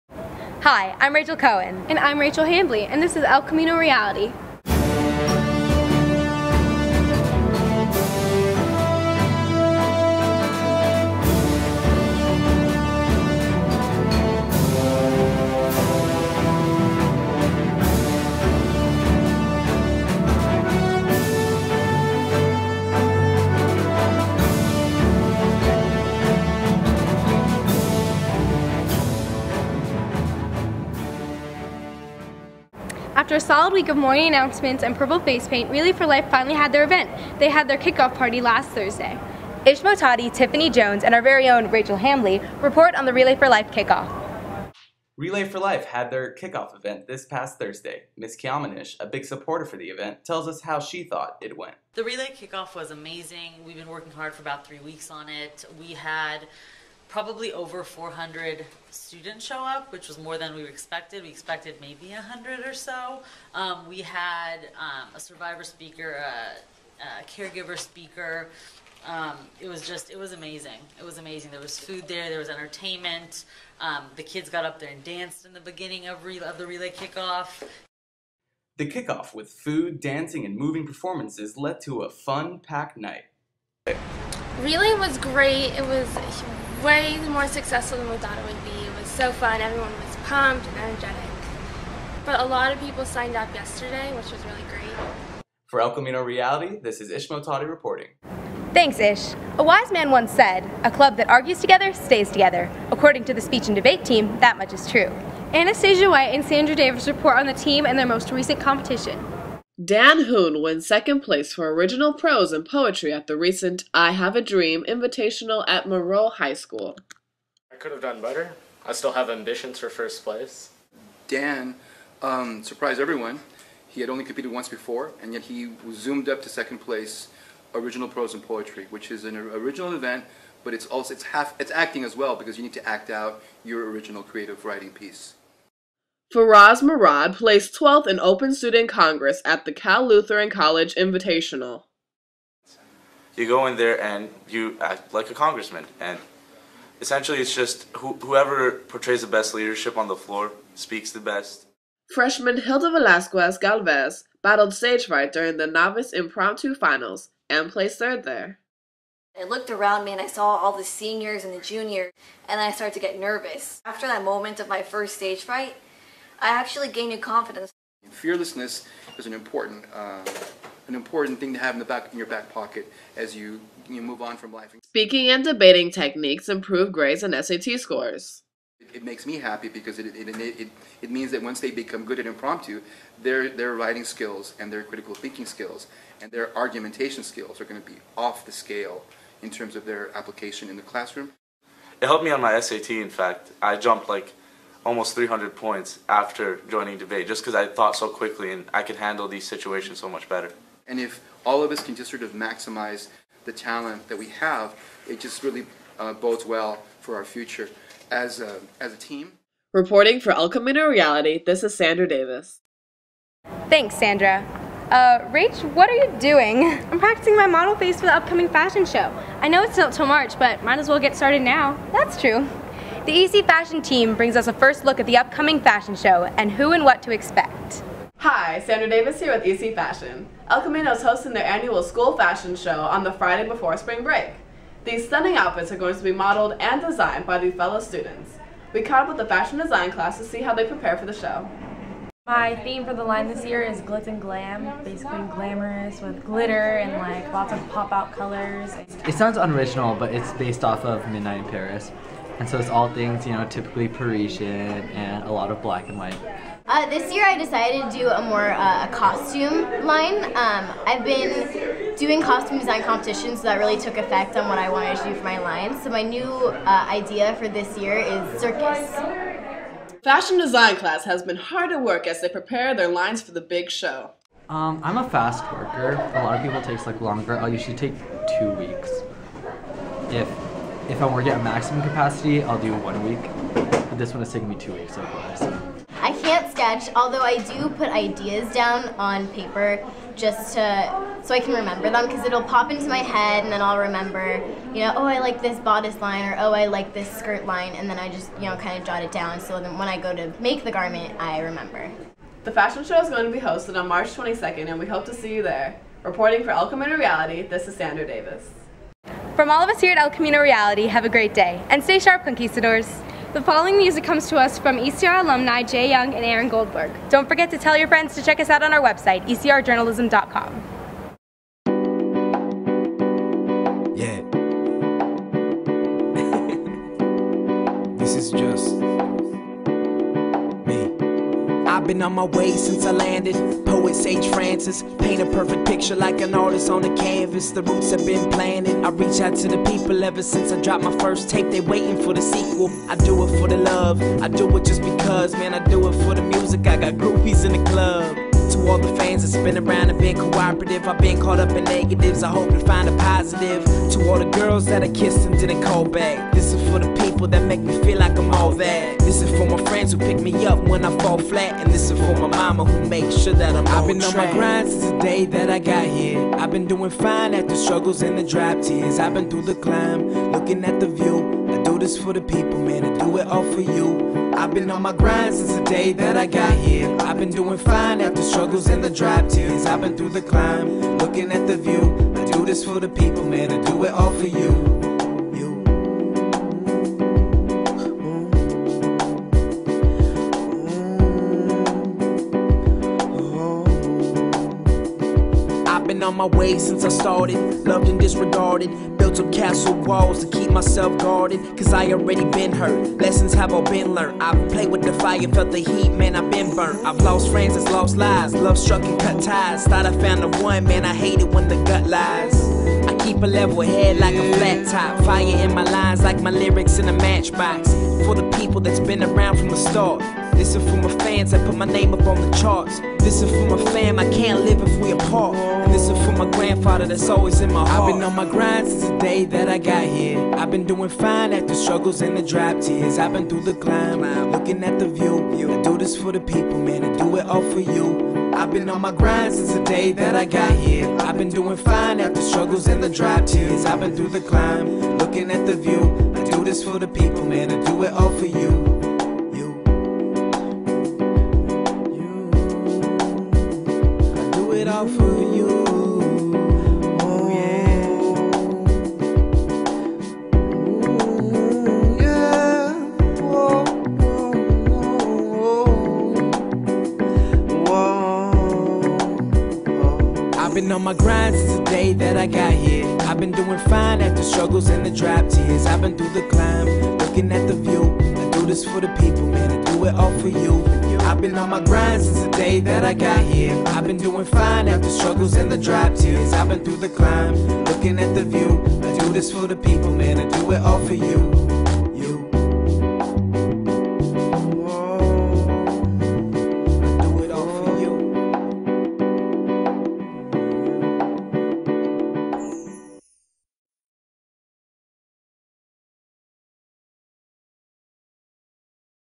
El Camino Reality - Season 3 Episode 20
"El Camino Reality" is a webcast news show produced by the CEA's broadcast journalism class at El Camino Real Charter High School in Woodland Hills, CA, under the guidance of Kimberly Messadieh. This episode features the very successful Relay for Life kickoff party, the speech and debate team, the upcoming fashion show and a song produced by ECR alumni, Jacob Yung and Aaron Goldberg.